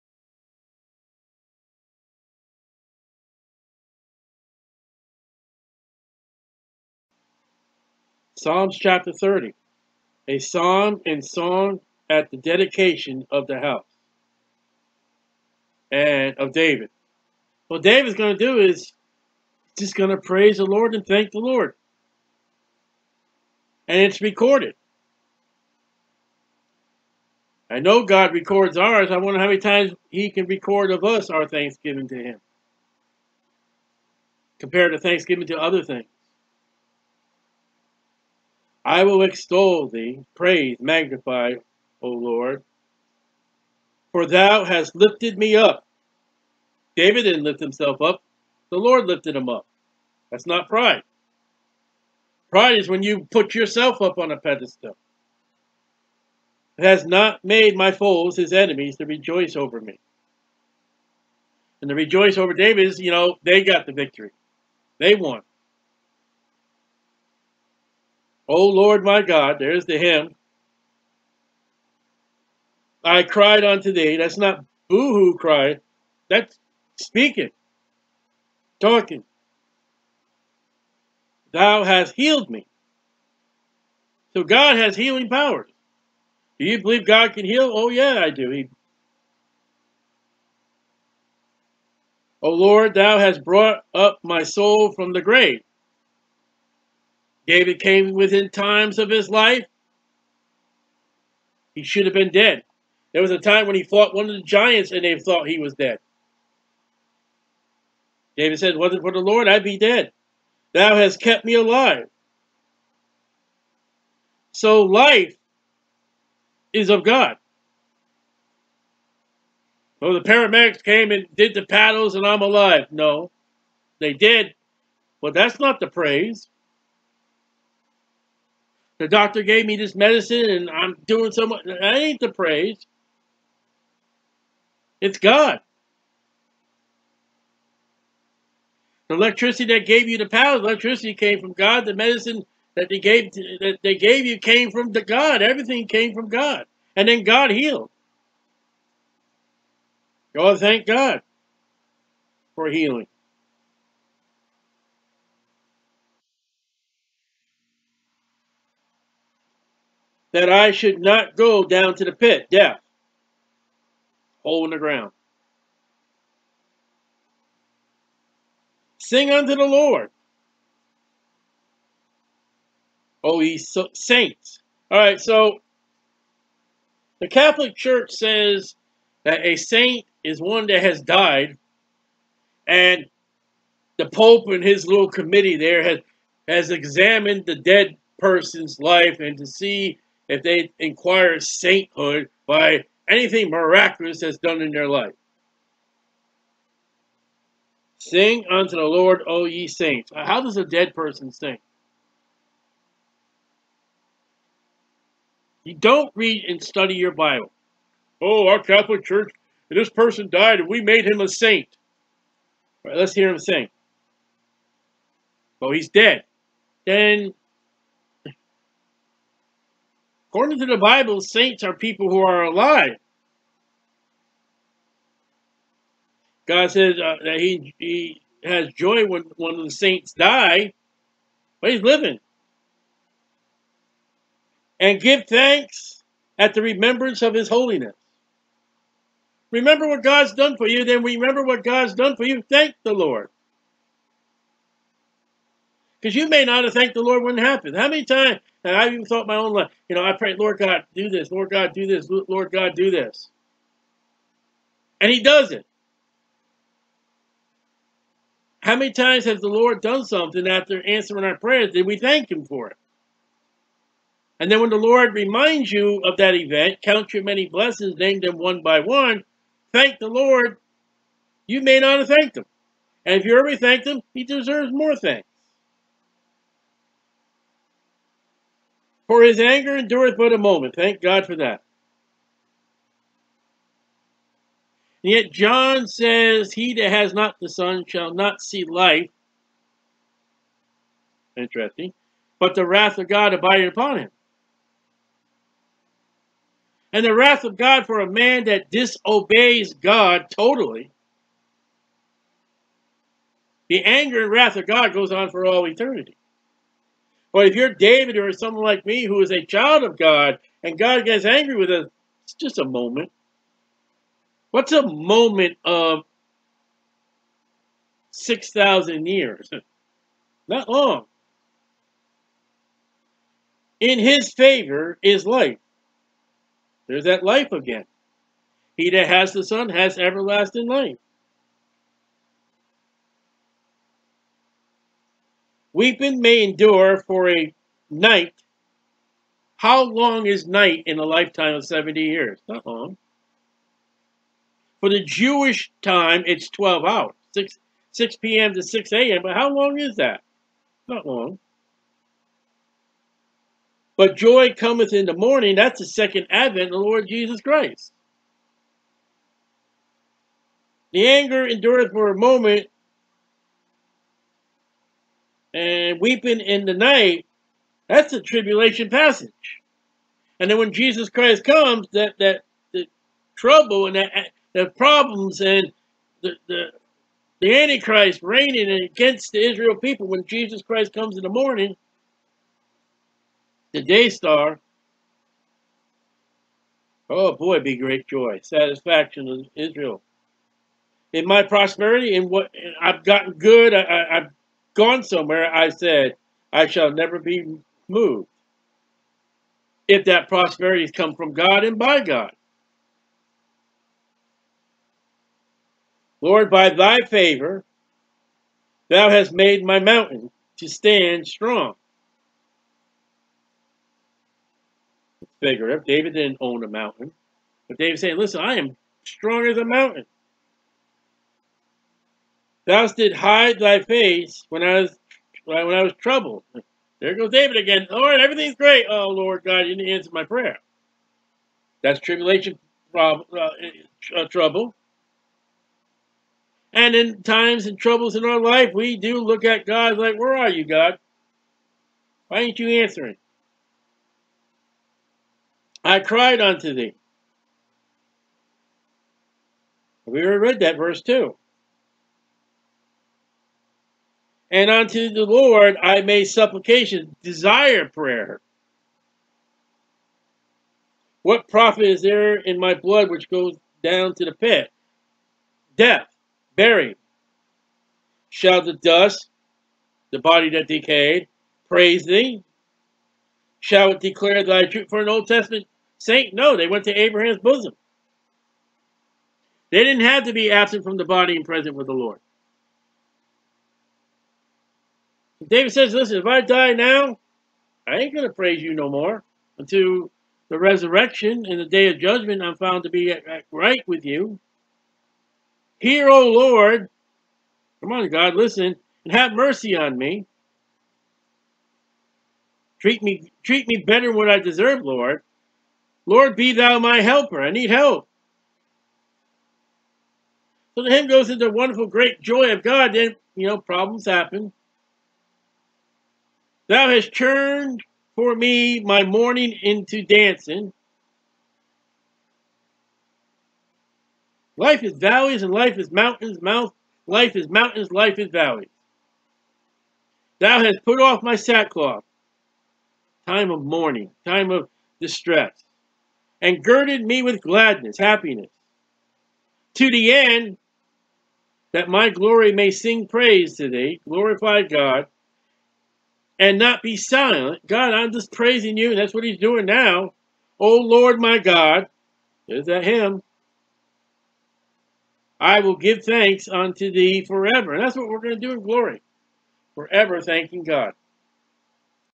David Just Thanking His God Psalm 30
David is dedicating his house to God in joy. He is so pleased and happy with God that he has to express it in words of a psalm.

Jesus is worthy of all praise.